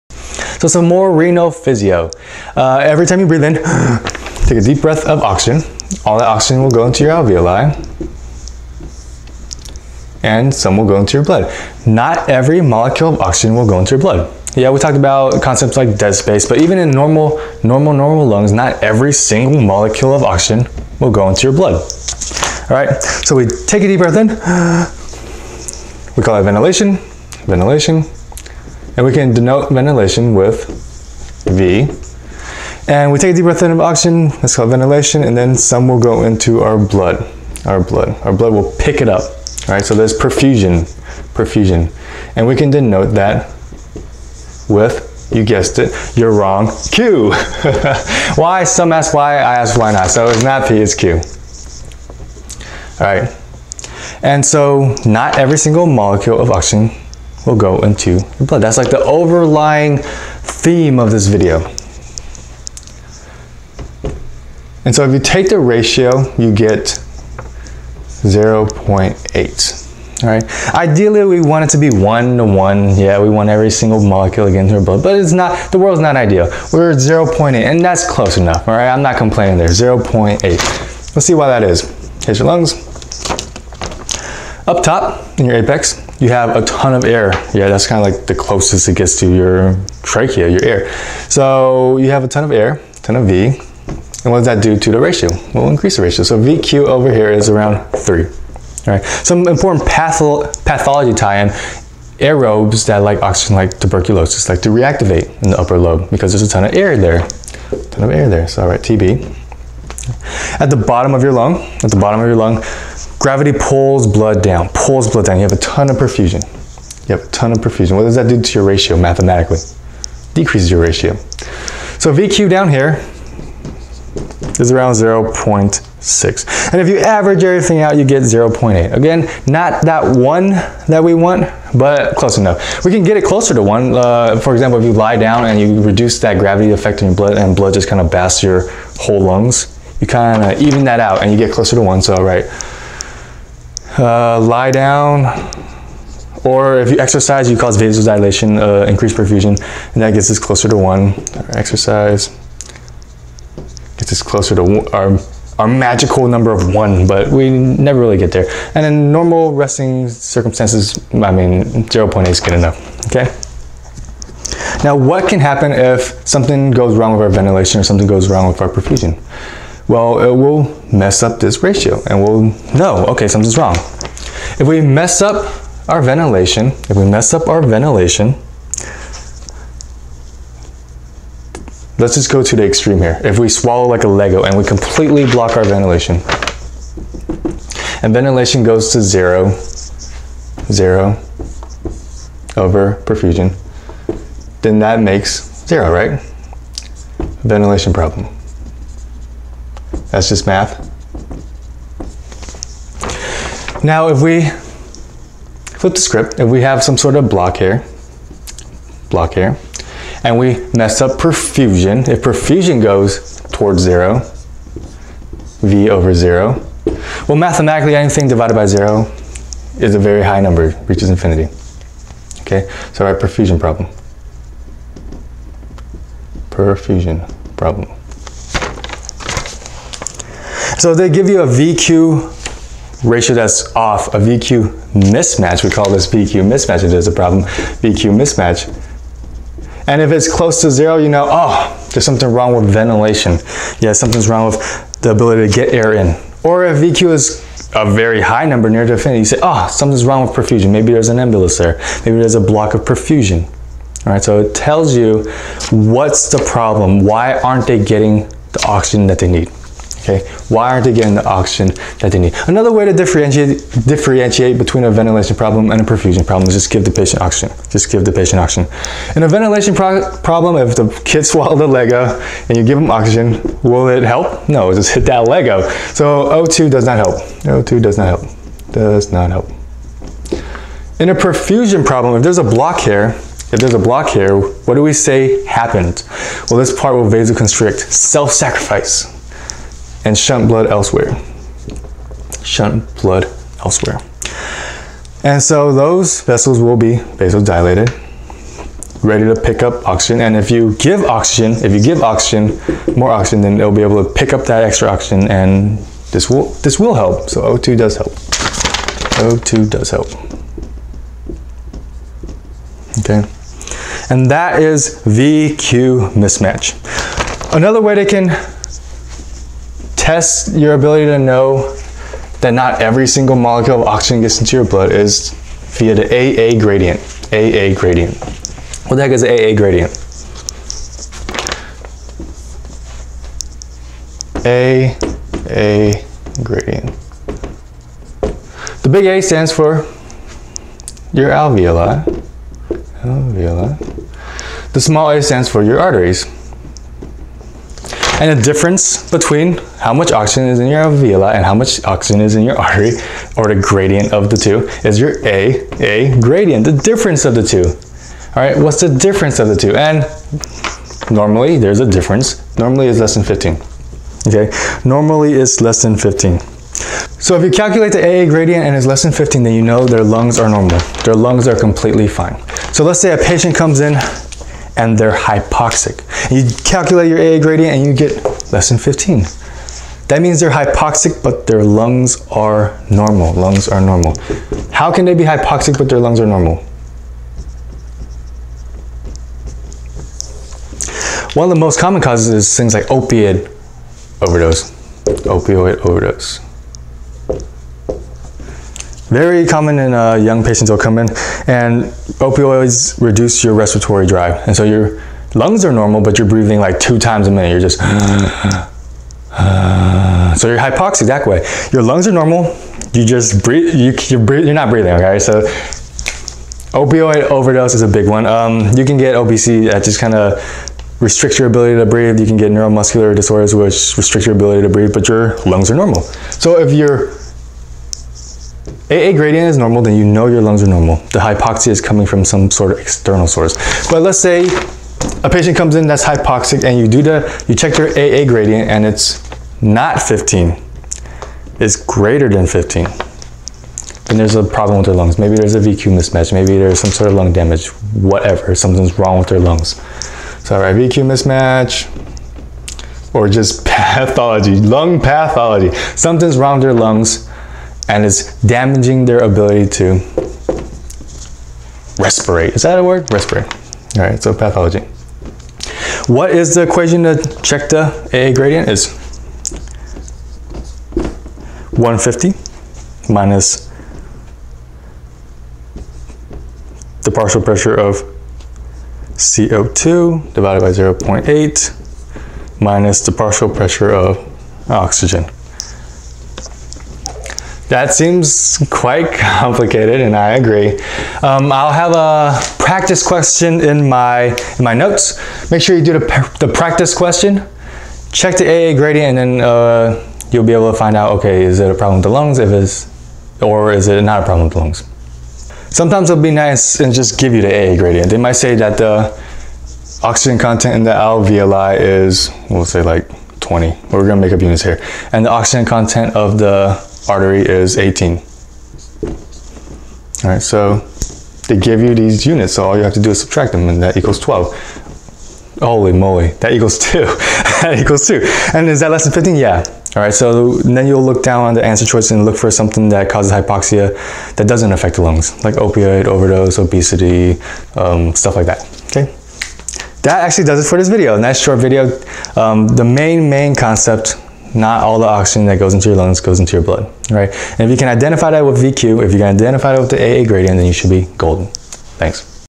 USMLE Respiratory 4: Physiology - V/Q Mismatch and A-a Gradient
Follow along with First Aid, or with my notes which can be found

More lung physiology! The big overarching message today is that not every single molecule of oxygen that you breathe in will go into your blood! Now there are several ways we can look at this. The first way is measuring VQ ratio. Ventilation is measured as V, and perfusion is measured as Q. Now ideally, we'd want it to VQ = 1, but instead it's 0.8. Now the apex of the lung has more ventilation and VQ is 3. At the base, it is 0.6 and thus the average is about 0.8. You can get this closer to 1 by exercise or by lying down. Now how can lung pathology affect our VQ? Let's take it to the extreme. What if there is no ventilation? Then V = 0 and thus V/Q is close to 0. This does not respond to oxygen therapy.  If perfusion reaches 0, then P = 0 and thus VQ = infinity. This does respond to oxygen therapy.

Now another way to illustrate this point is via the A-a gradient. This is the difference between the oxygen content in your alveoli and your arteries! Normally the gradient is  less than 15 meaning the lung is normal. Thus if a pt has hypoxemia or hypoxia but they have a normal A-a gradient, then the problem is outside the lungs! Make sure you look at my notes to see the equation and a practice question!

Done!